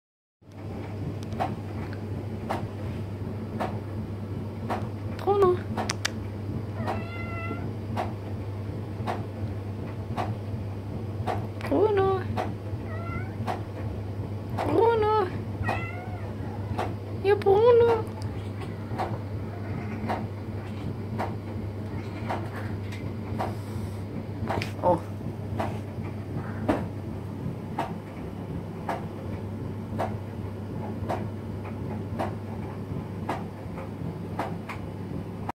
🐱 Cute black kitten annoys cat 🐱
A curious kitten annoys a cat